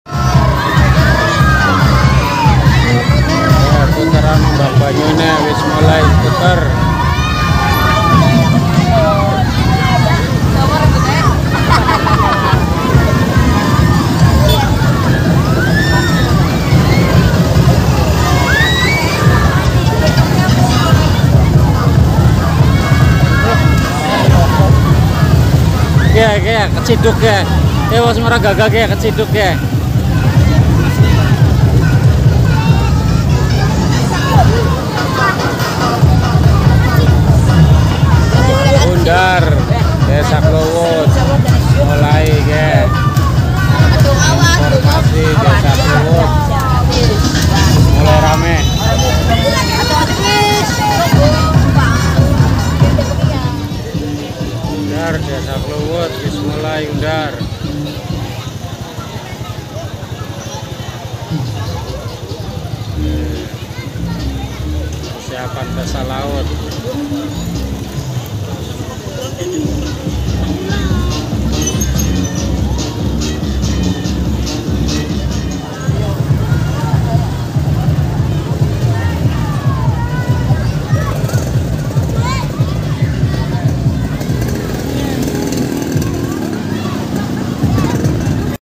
pasar malam pesta laut desa kluwut
pasar malam menyambut pesta laut di desa kluwut kecamatan bulakamba kabupaten brebes.